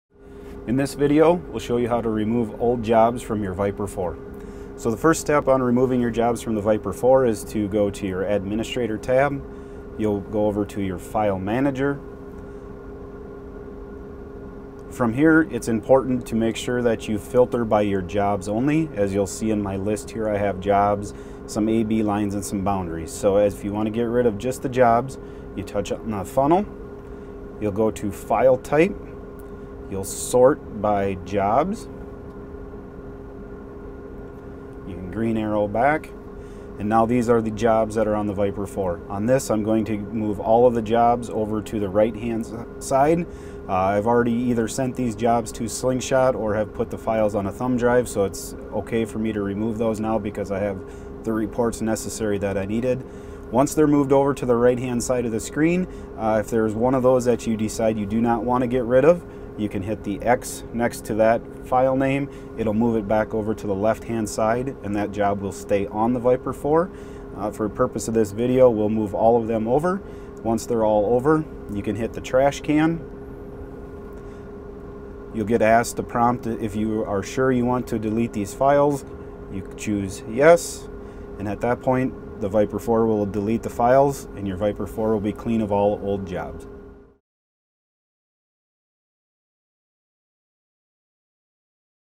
Remove Old Jobs on Viper 4+ Display
Understand how to delete files and use filters to select the correct file types to delete.